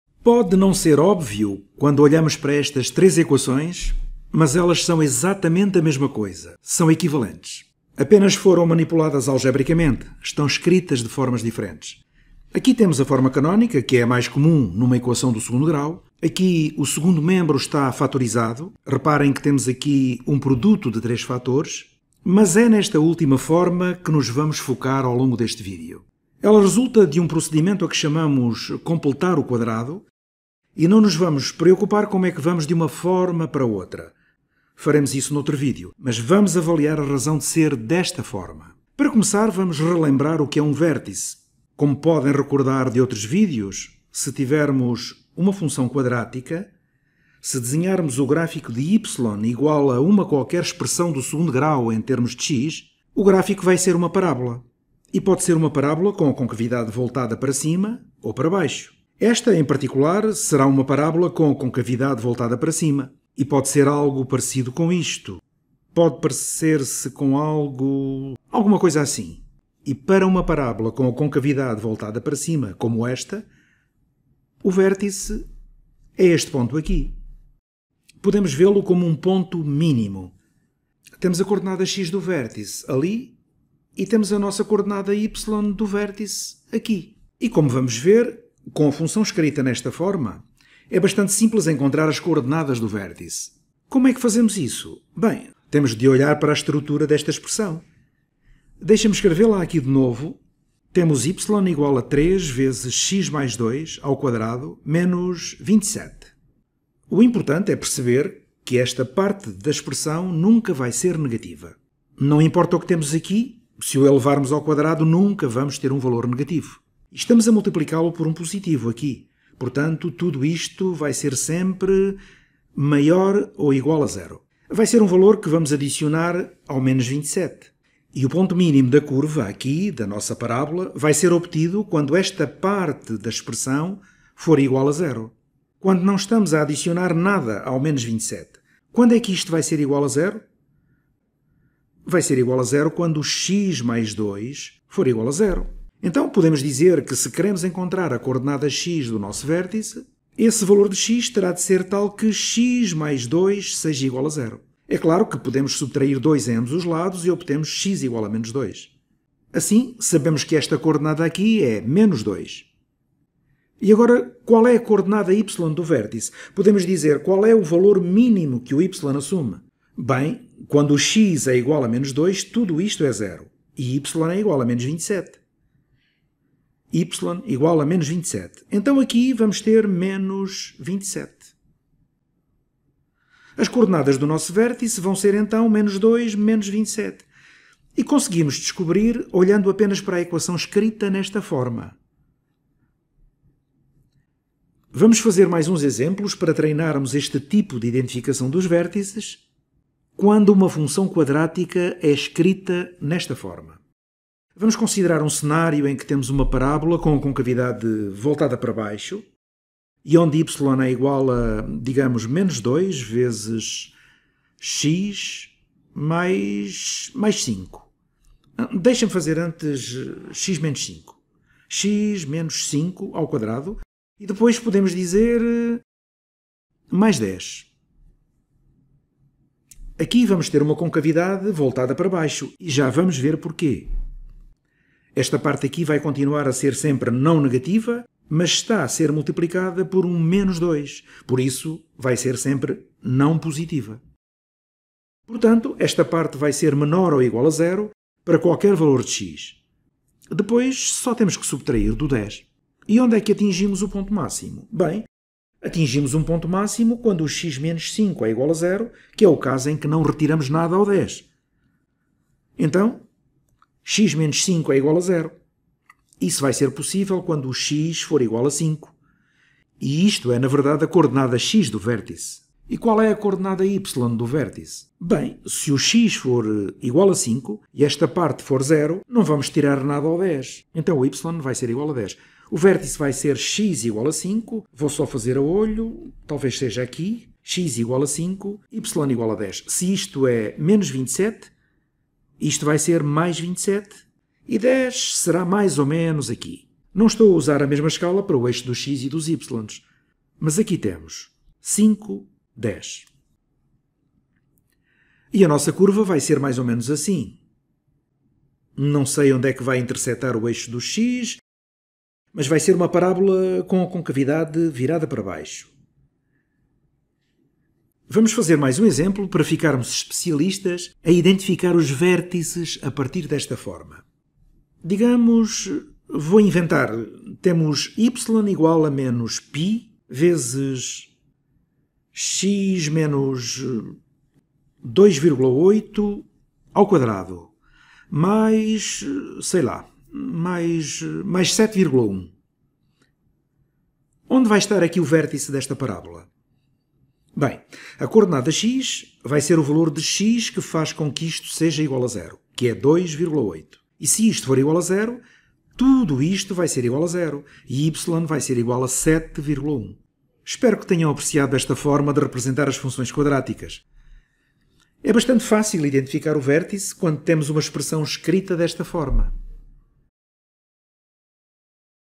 “Completar quadrados” em equações do 2.º grau. Vértices de parábolas (10º ano)
Vídeo original: Vertex form introduction